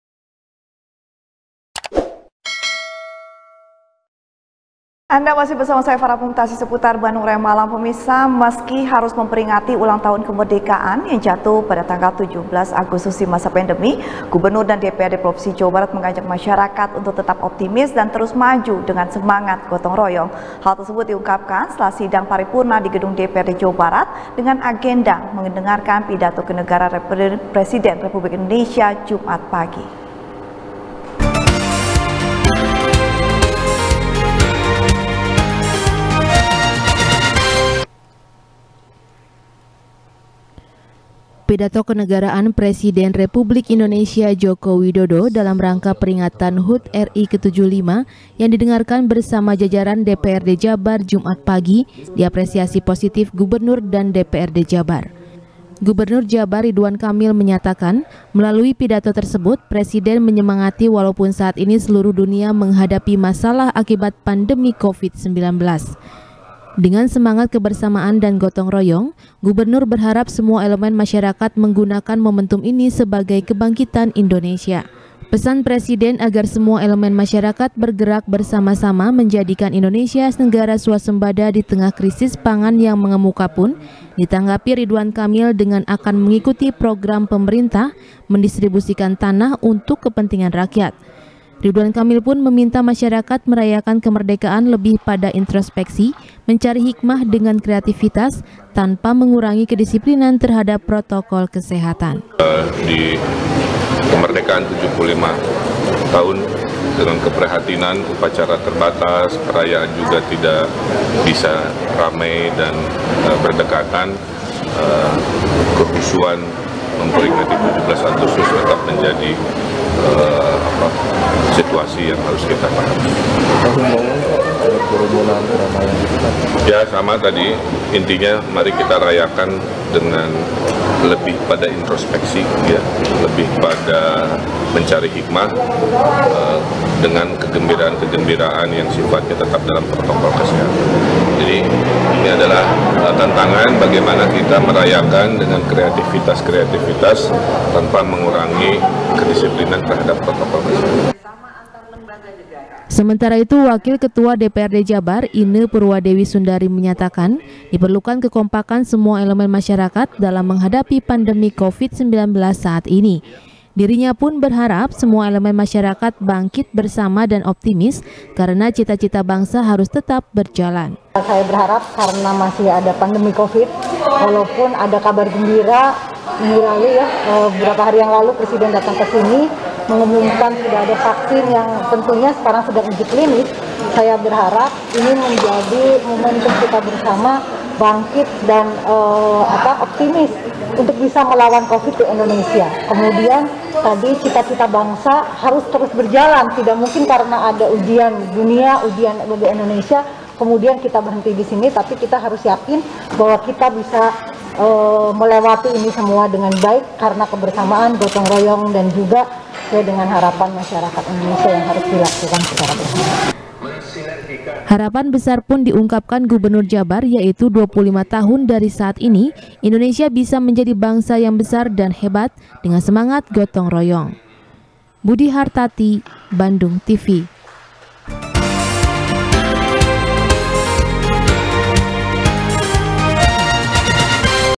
17-an Di Masa Pandemi, Gubernur Dan Dprd Ajak Masyarakat Optimis | SBR | BANDUNGTV
SBR | BANDUNGTV | FARHAT MUMTAZ | Meski harus memperingati ulang tahun kemerdekaan yang jatuh pada 17 Agustus di masa pandemi, Gubernur dan DPRD Jawa Barat mengajak masyarakat tetap optimis dan terus maju dengan semangat gotong royong. Hal tersebut diungkapkan setelah sidang paripurna di gedung DPRD Jabar, dengan agenda mendengarkan pidato kenegaraan Presiden Republik Indonesia, Jumat pagi.